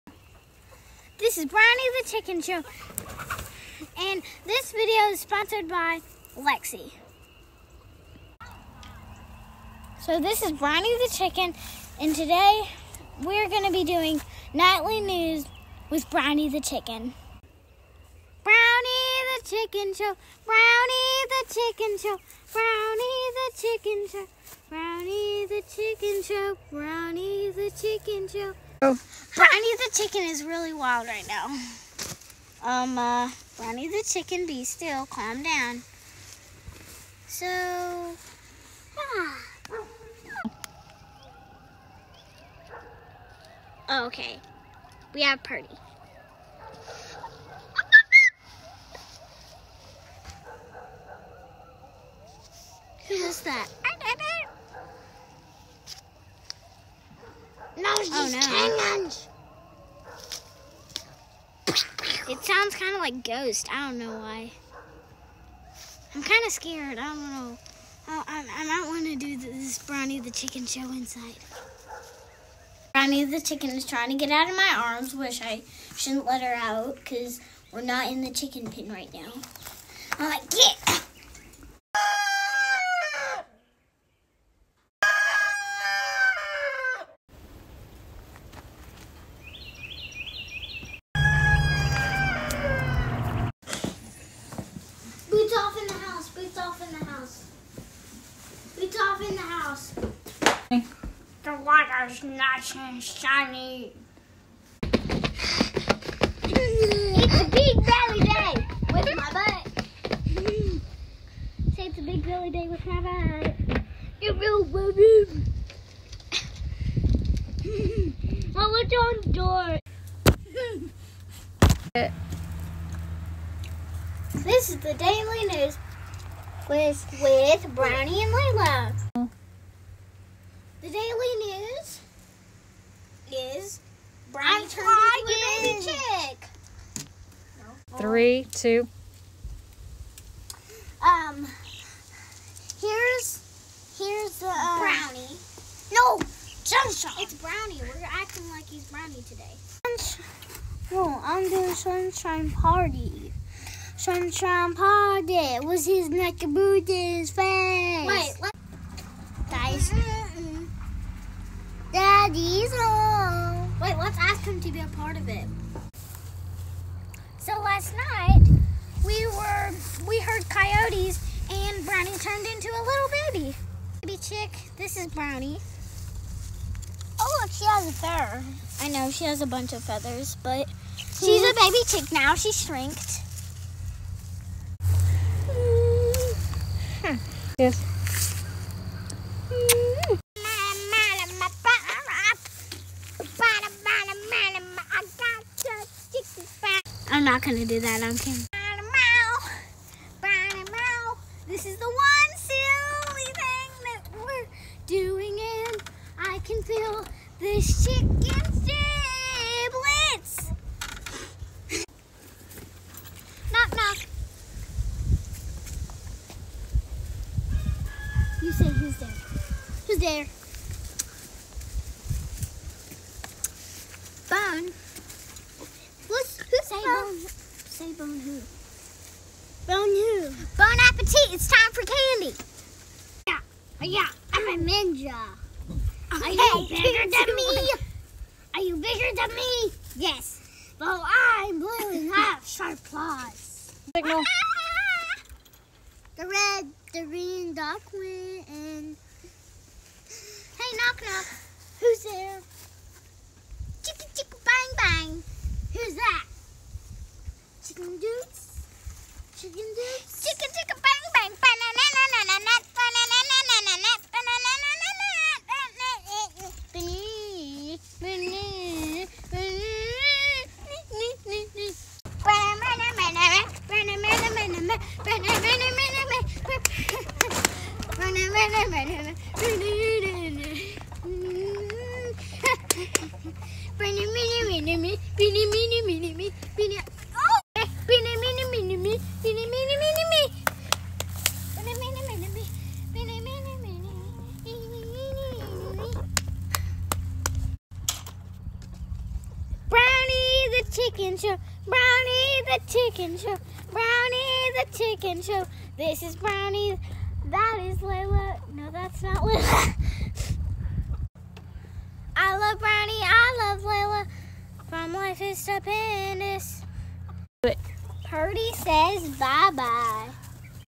Brownie the Chicken Show, Episode 4: The Nightly News Fail
In this episode of Brownie the Chicken Show, the Nightly News is interrupted by coyotes, Brownie shrinks and the kids have jokes.